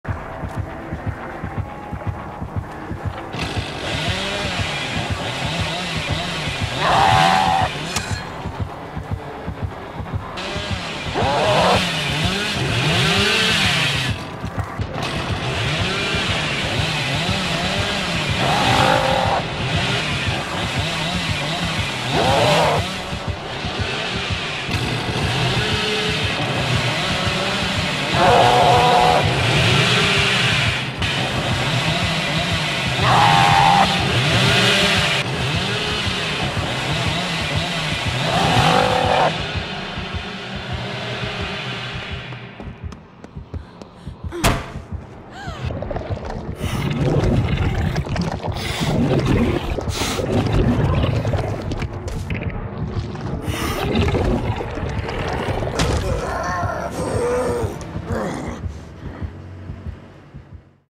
You Can Counter These Killers By Crouching...
This bug has been in the game for a couple of months now however has only recently started to surface. When you crouch slightly below either 3 of these killers as soon as they use their power they'll slide right past you allowing you to escape unharmed countering them by crouching...